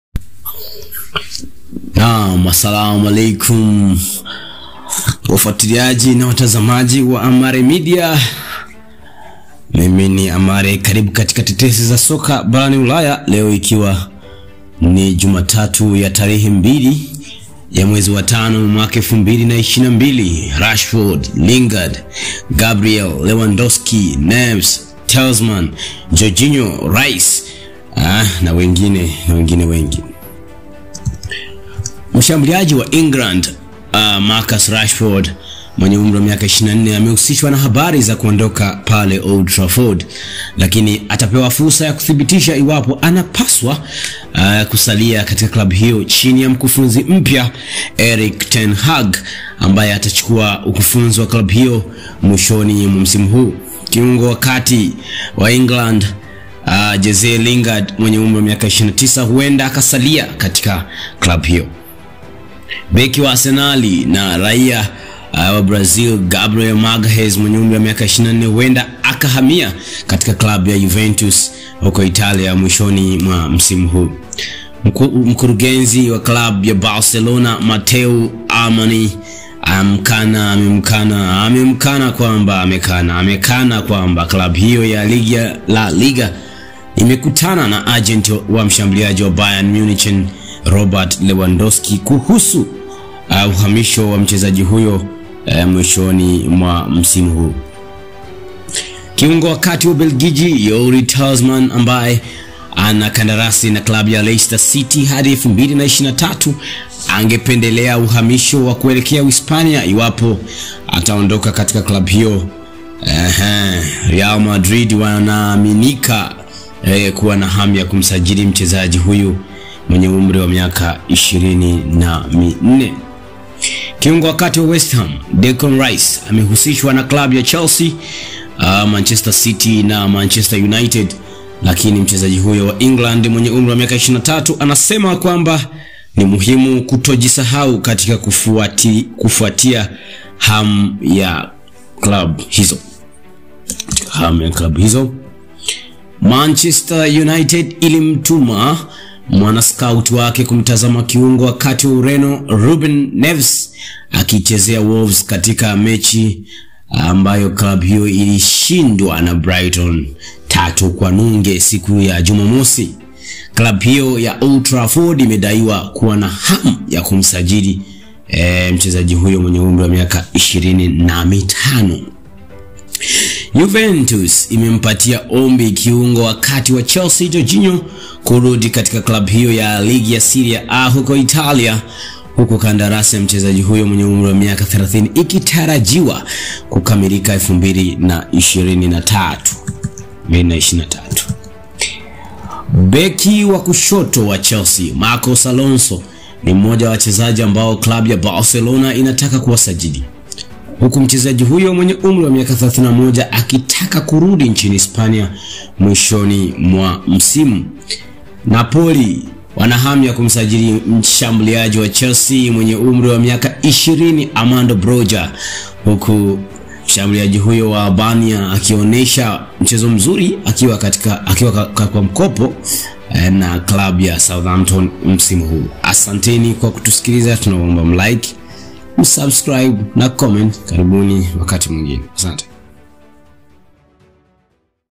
TETESI ZA SOKA BARANI ULAYA 3/5
Tetesi za soka Ulaya Jumatatu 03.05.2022: Rashford, Lingard, Gabriel, Lewandowski, Neves, Tielemans, Jorginho, Rice